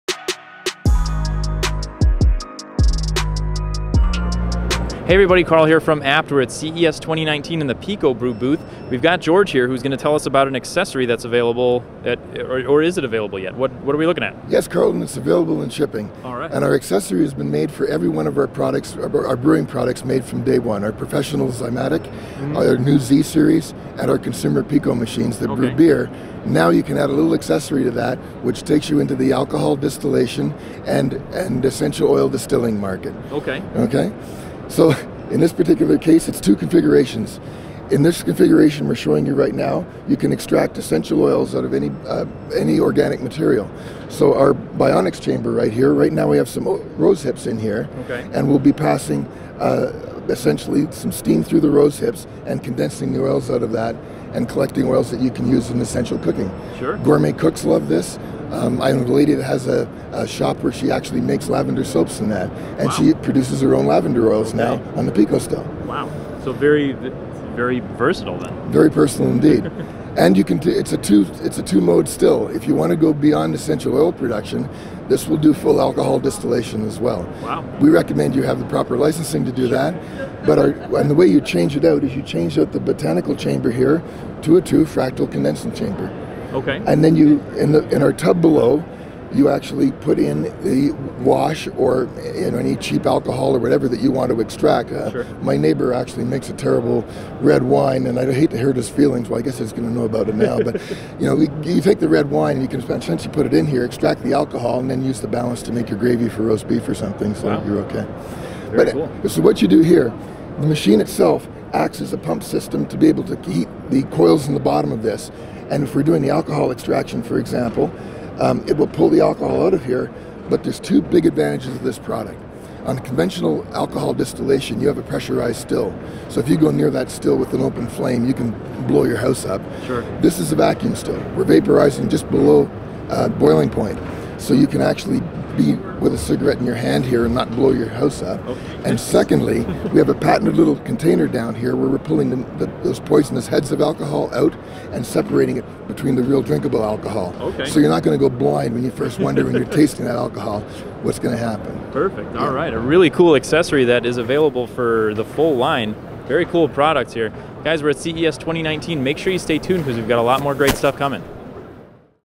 CES 2019 - PicoBrew PicoStill
We're at CES 2019 taking a look at the PicoBrew PicoStill. For the latest at CES 2019, check out The Bolt